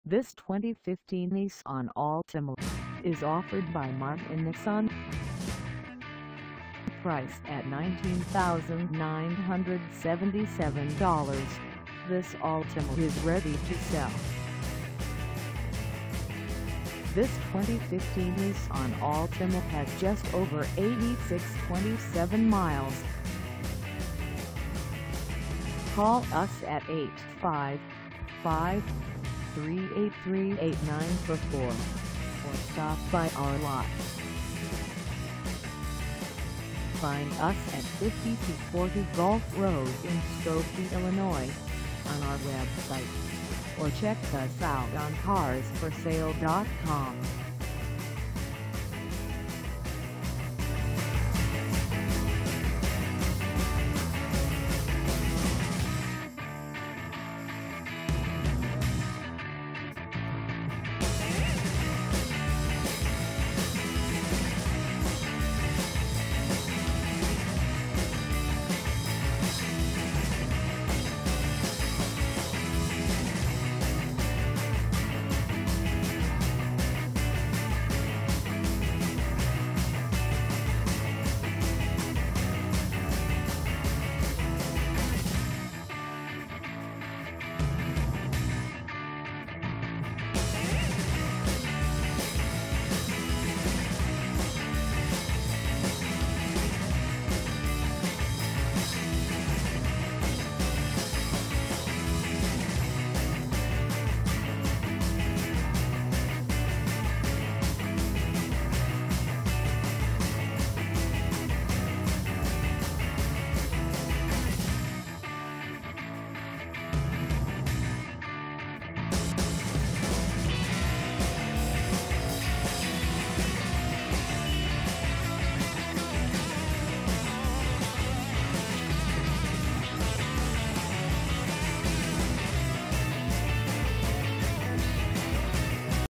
2015 Nissan Altima 2.5 S 4dr Sedan for sale in Skokie, IL 60
This 2015 Nissan Altima 2.5 S 4dr Sedan is for sale in Skokie, IL 60077 at Martin Nissan.

Engine: 2.5L I4
Transmission: CVT
Color: Gun Metallic
Mileage: 8627
Contact Martin Nissan 5240 Golf Road Skokie, IL 60077 to test drive this 2015 Nissan Altima 2.5 S 4dr Sedan today.